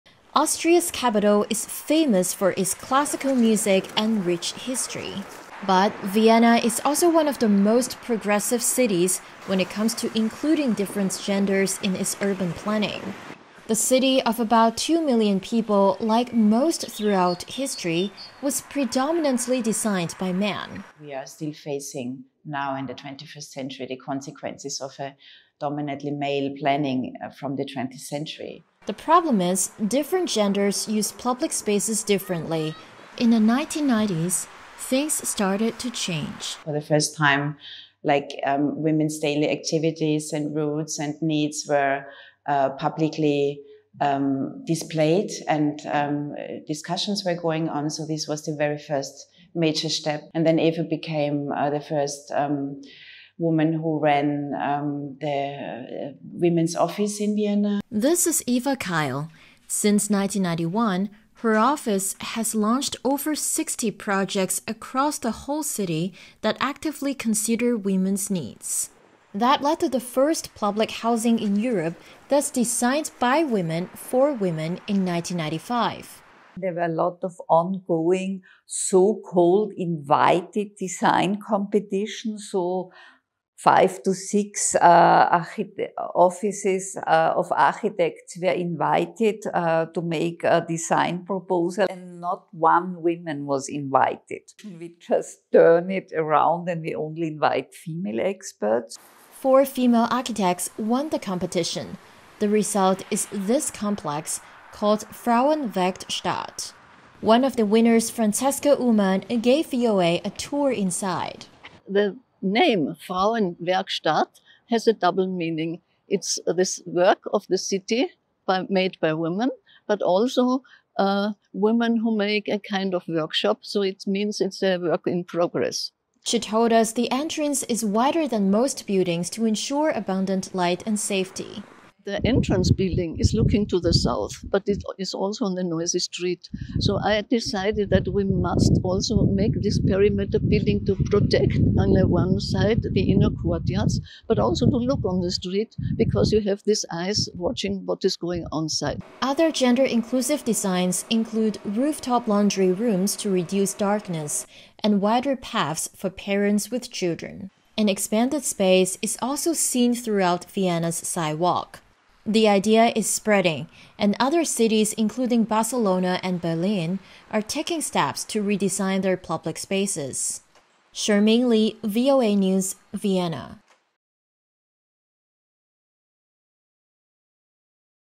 Vienna Sets Trend in Gender-Friendly City Planning | VOANews
Austria, has been ranked by The Economist as the most livable city in the world and it is also the pioneer of adopting a gender-inclusive urban design. For VOA, Chermaine Lee reports from Vienna.